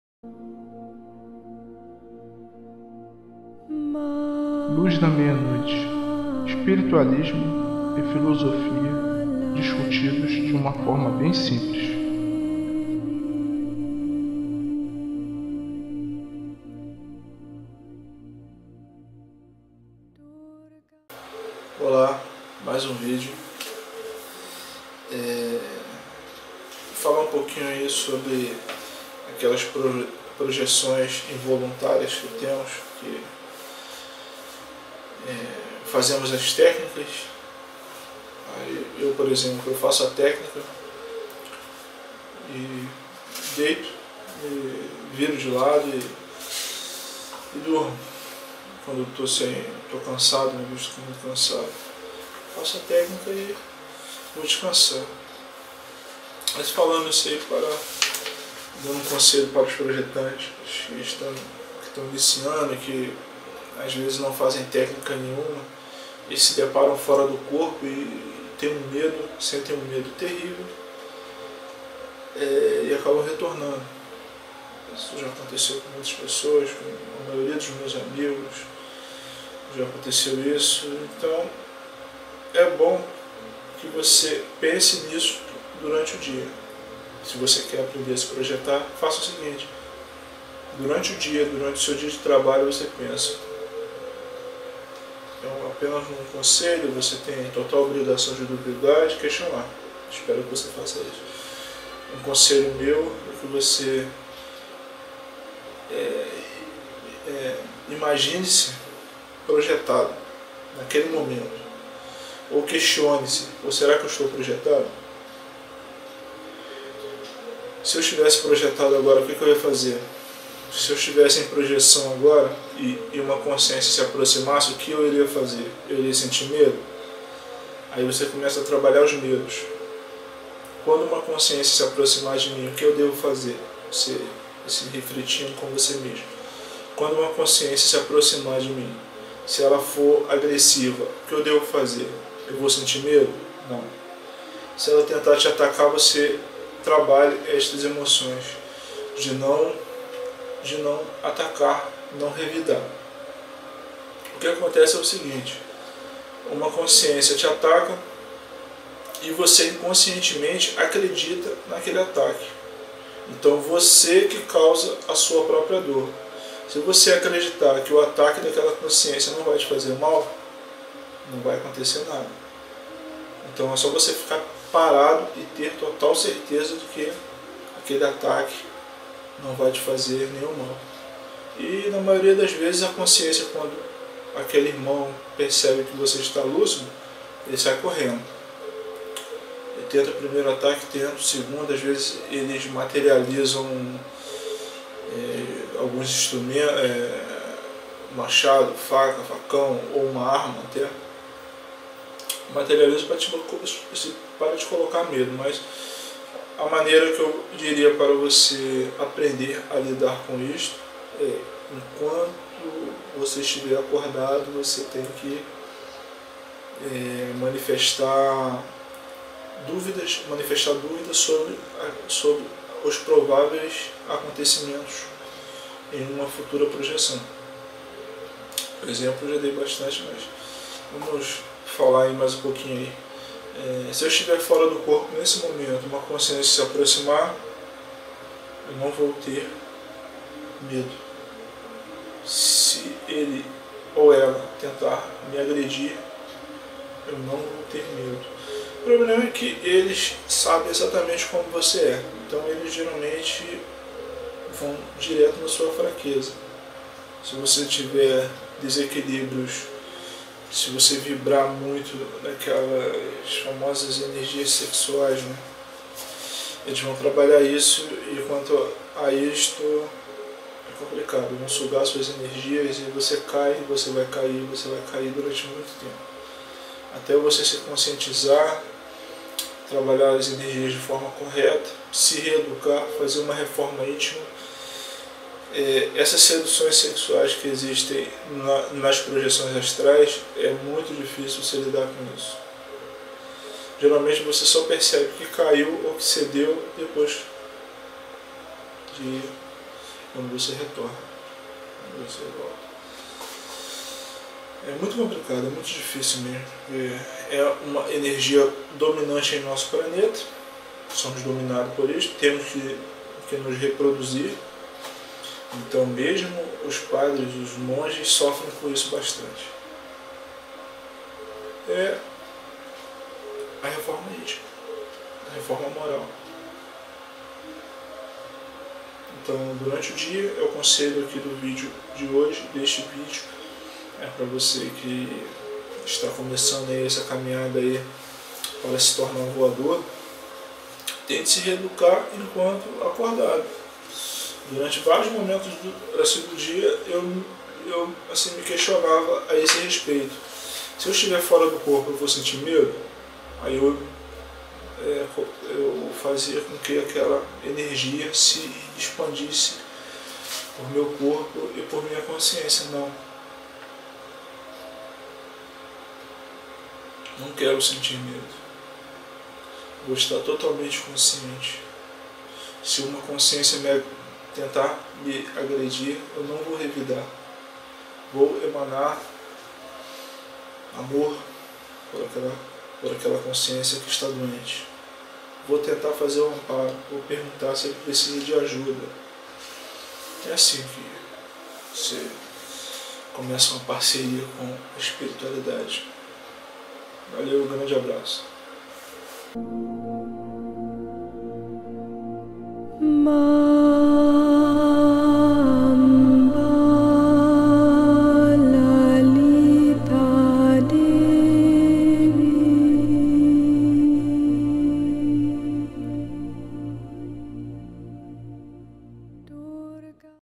Controlando o Medo - Projeção Astral Iniciante.
Luz Da Meia Noite: Espiritualismo e Filosofia Discutido de Uma Forma Bem Simples.